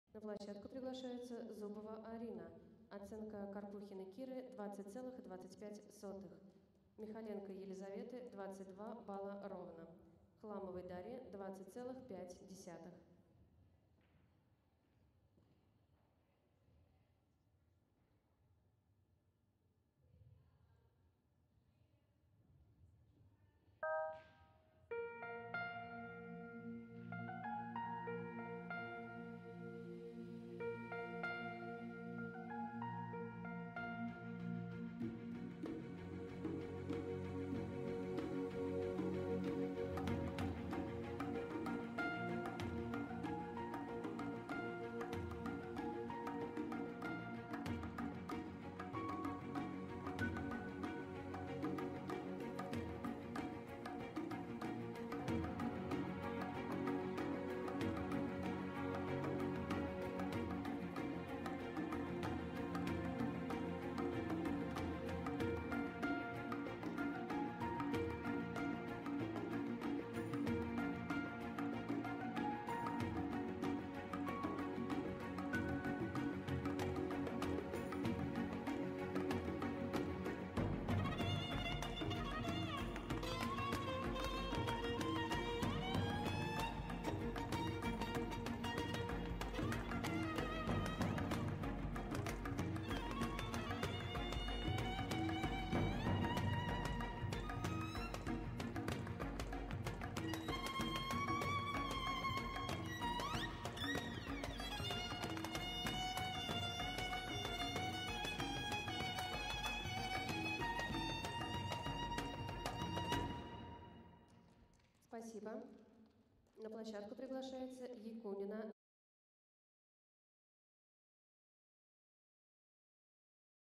Арина Зубова | Художественная гимнастика выступление
Выступление Арины Зубовой на всероссийских соренованиях МФСОО Рекорд, город Раменское.